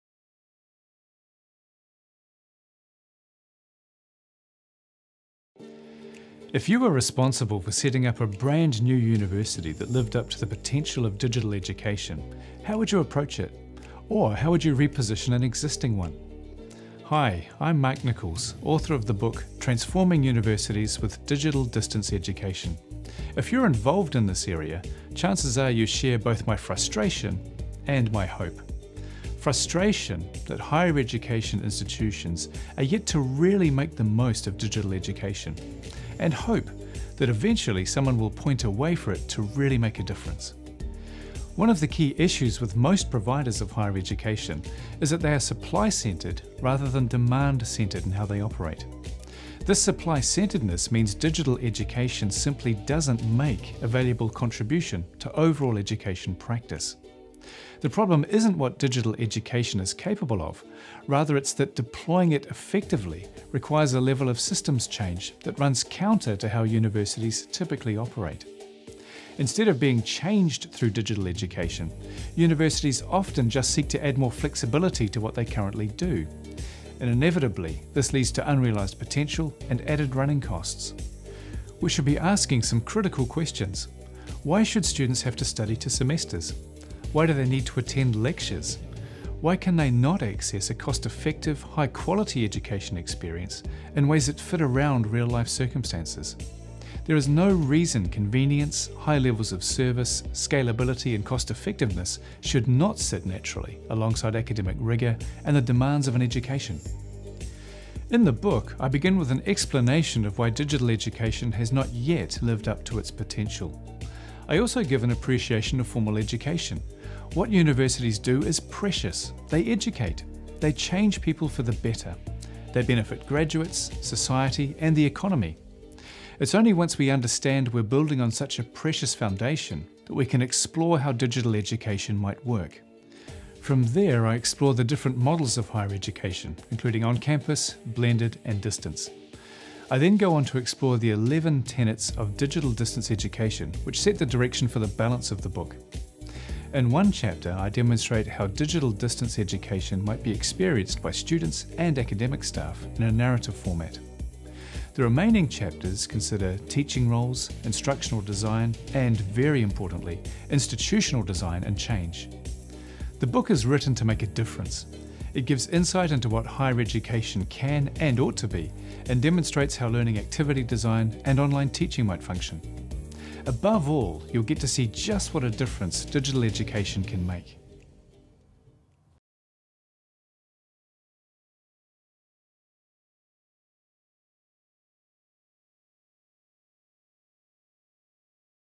Mark Nichols - Transforming Universities with Digital Distance Education
Transforming Universities with Digital Distance Education explores the ways in which higher education stakeholders can apply and leverage the benefits of online learning. Systems-wide access, scale and quality are achievable goals but require forms of teamwork and financial modelling beyond those at the instructor or programme level. This book’s organisational view tackles the systems and practices that will help senior managers and decision-makers guide an entire institution away from dysfunction—incremental progress, insufficient capacity, high costs and generic products—and towards the macro-level implementation and operations of effective online pedagogies.